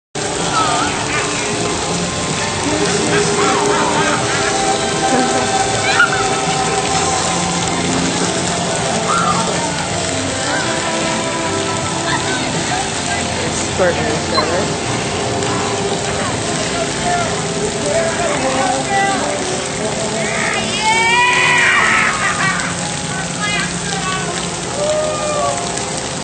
Bumper Boats at Frankie's Fun Park
Uncle tyler squirts everybody at the bumper boats